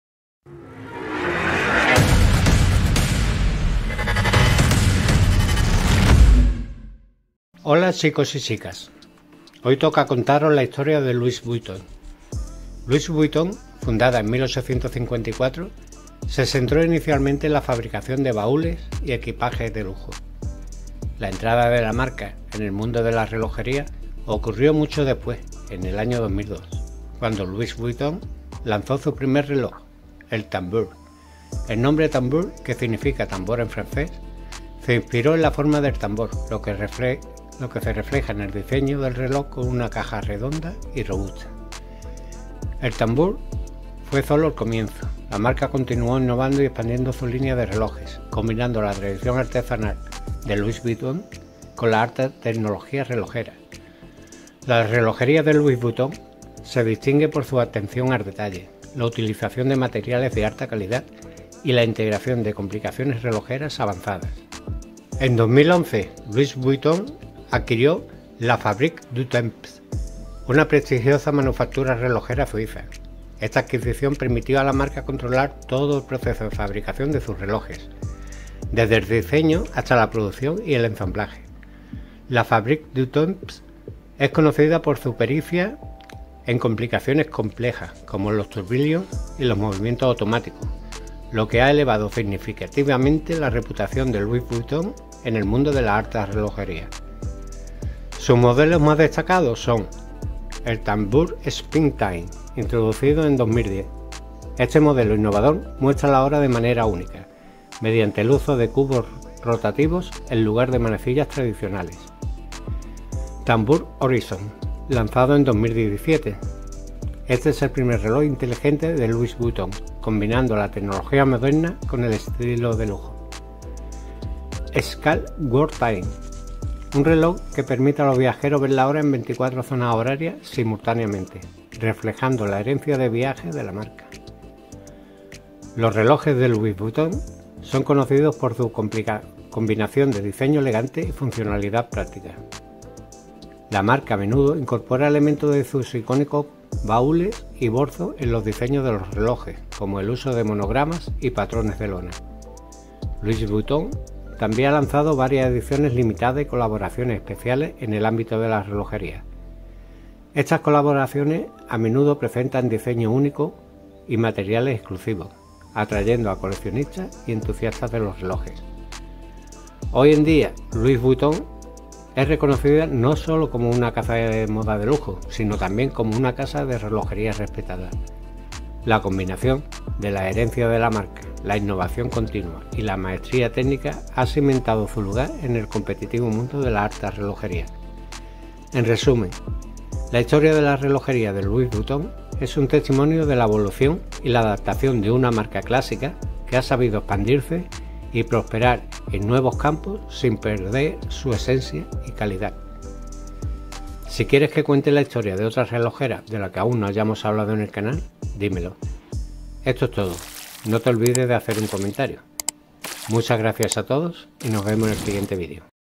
Breve historia de la relojera Louis Vuitton
Historia, brevemente contada, de Louis Vuitton, marca relojera de lujo que, aunque empezó tarde con los relojes, ha sabido hacer piezas únicas y bellas. Actualmente su sede se encuentra en París, Francia.